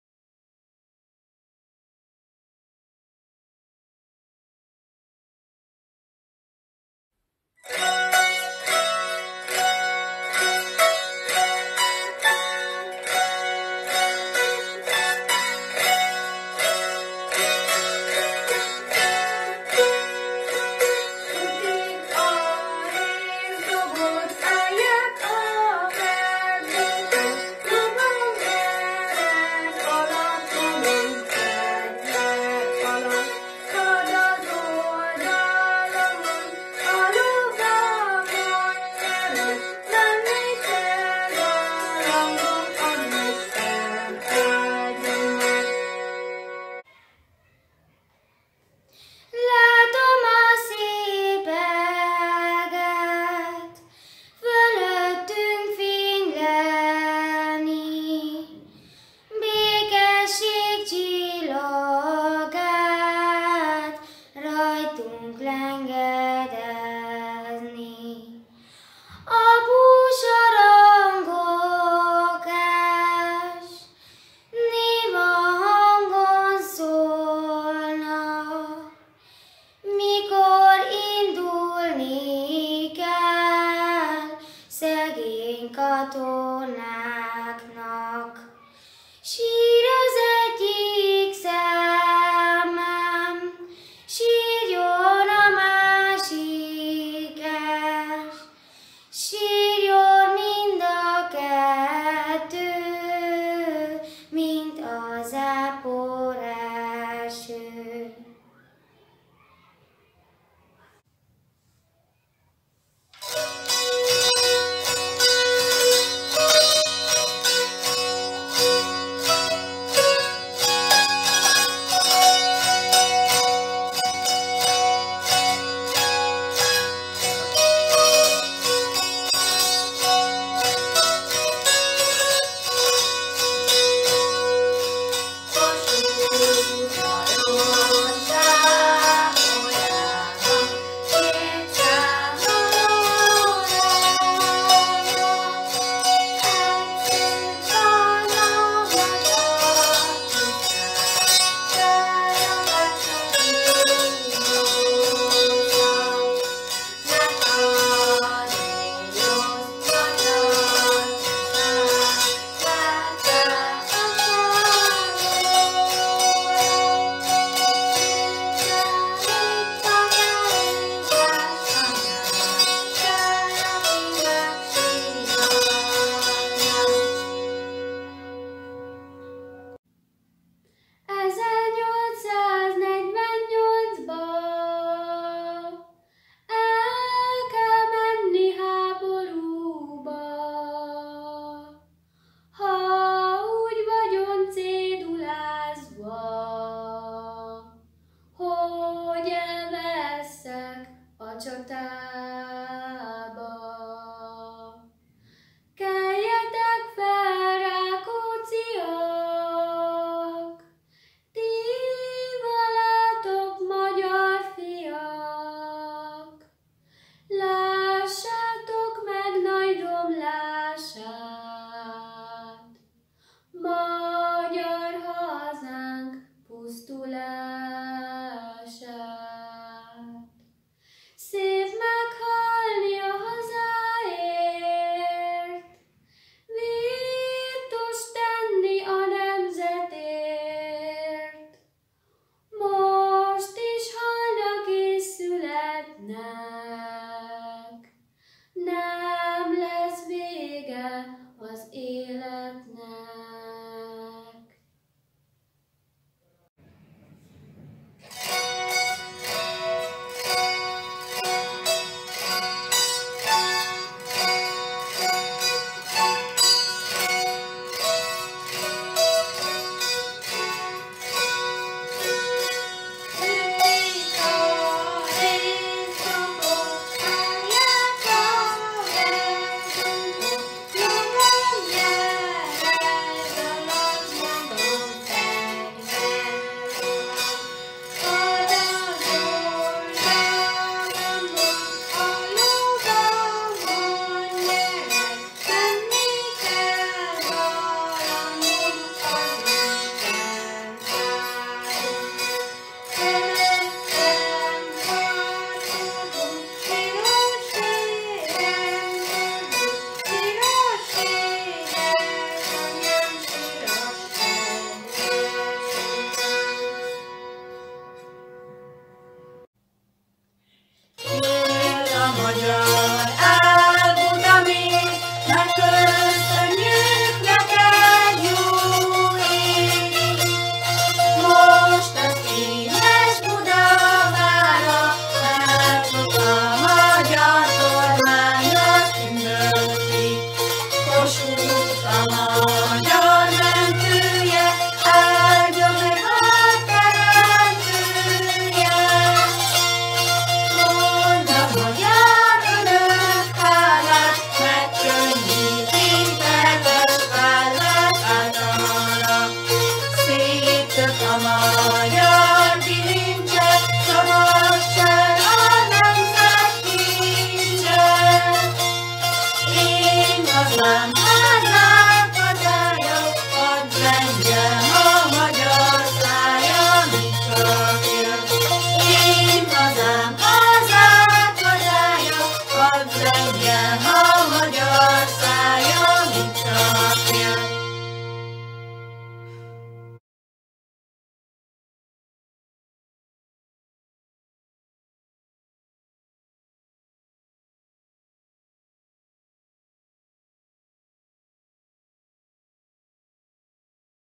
2021. Március 15-i műsor / Nyíregyháza-Eötvös / Dr. Szabóné Bugán Ágnes
Kedves Béla!
Csatolva küldök egy videót, amelyet a március 15-i ünnepség alkalmából készítettünk a szereplésre jelentkező gyerekekkel.